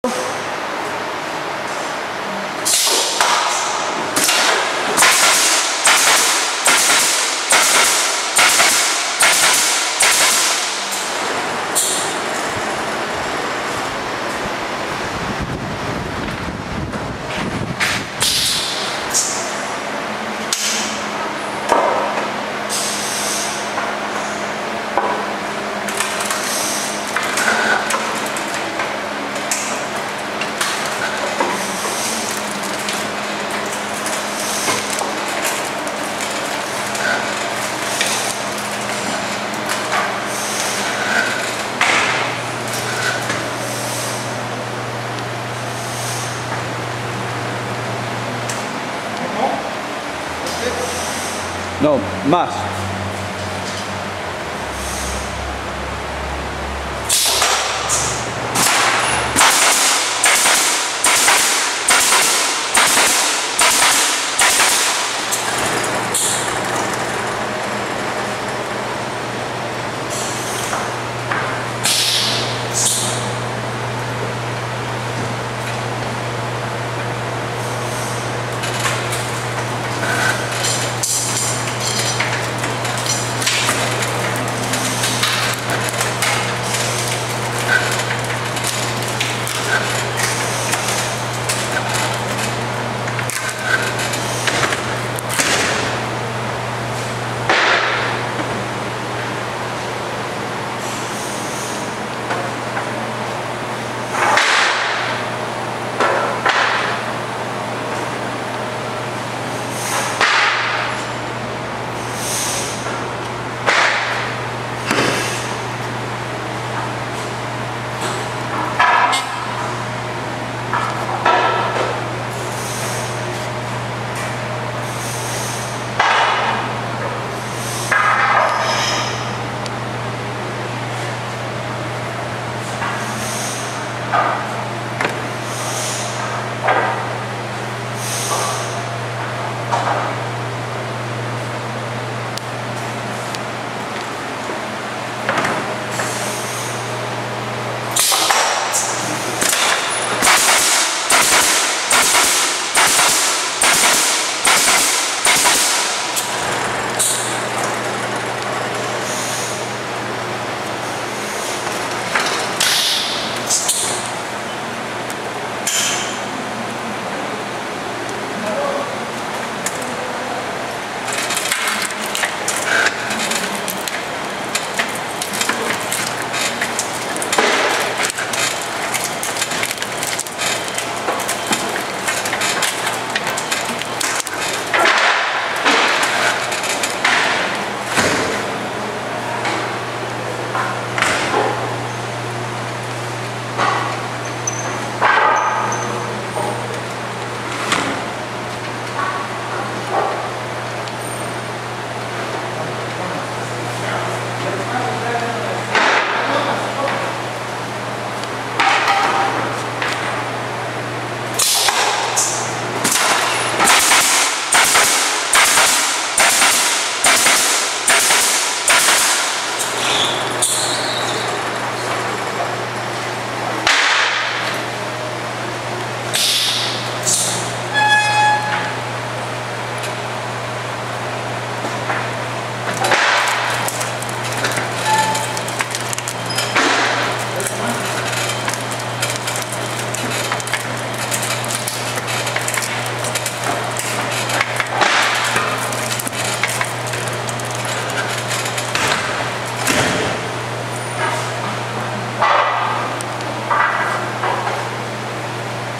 **NEW** Pallet Chief Nail Clincher
A component of our Block Pallet System and Deck Mat Nailer.  Works great for crates too.